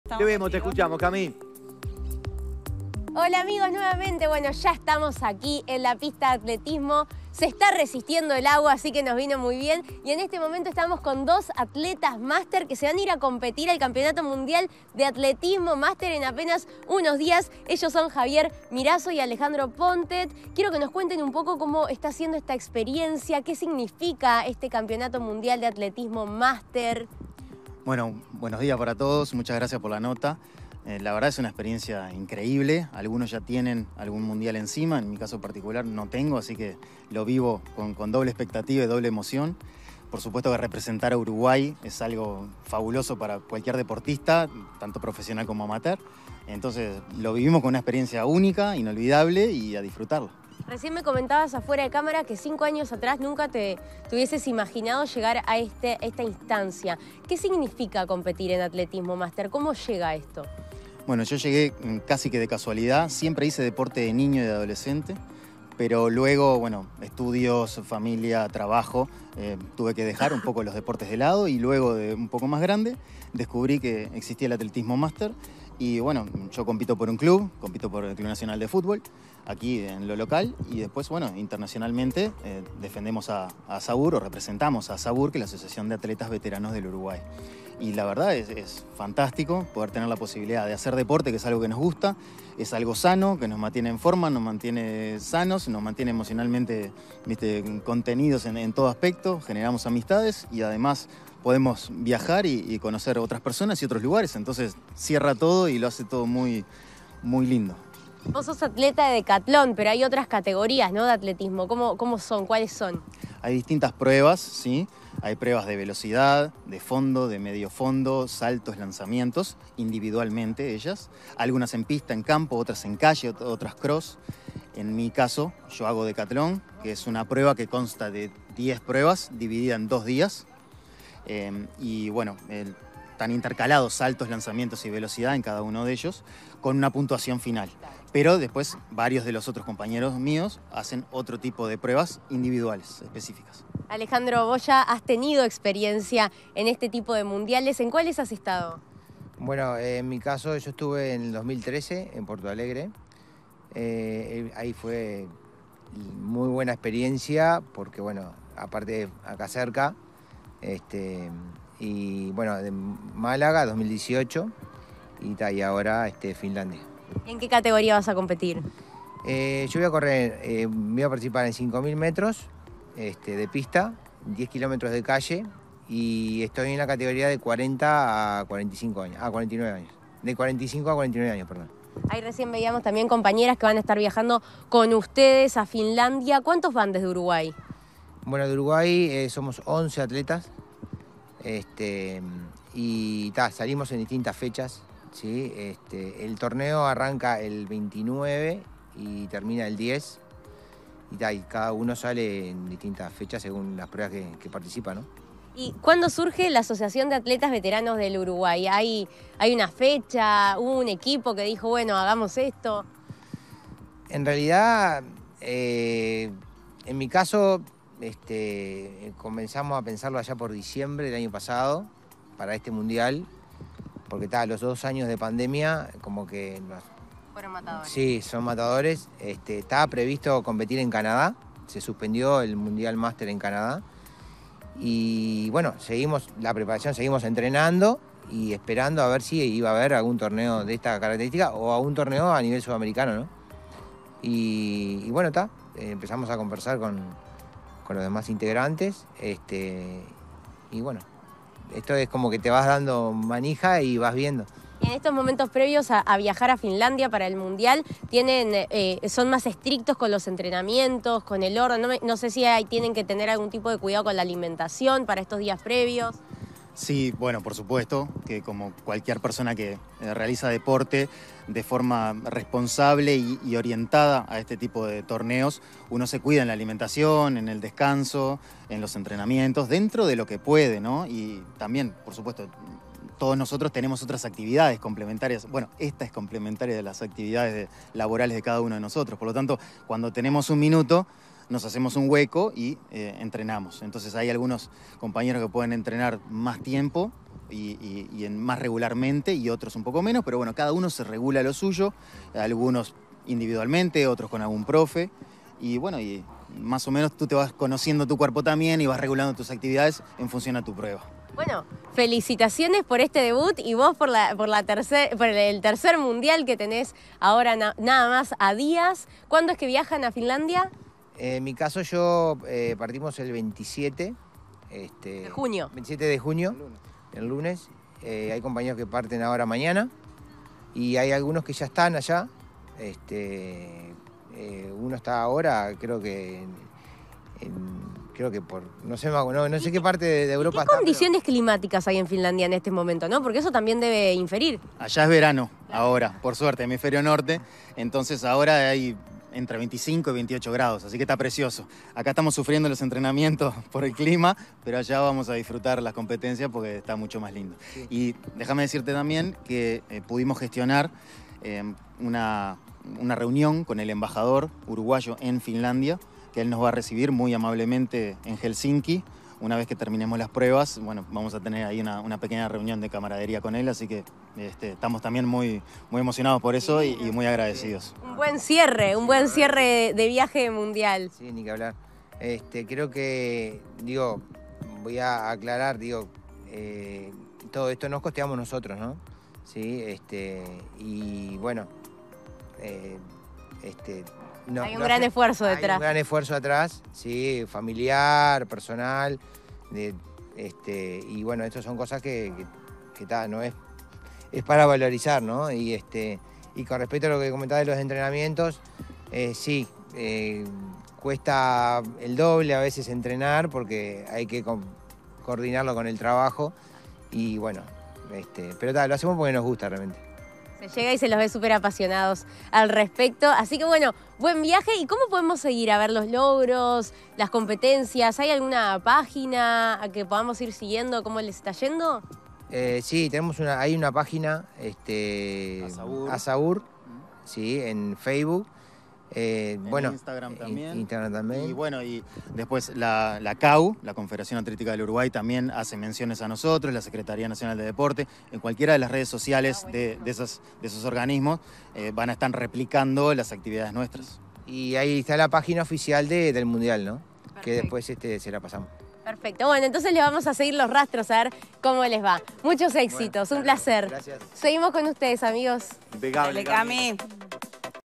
Atletas uruguayos representarán al país en el Campeonato Mundial de Atletismo Máster en Finlandia
Entre el 29 de junio y el 10 de julio se estará desarrollando el Campeonato Mundial de Atletismo Máster en las tierras finlandesas de Tempere, y el país será representado por los atletas uruguayos Javier Mirazo y Alejandro Pontet. Este viernes Cami conversó con los atletas para conocer más sobre esta experiencia y lo que significa competir en atletismo máster.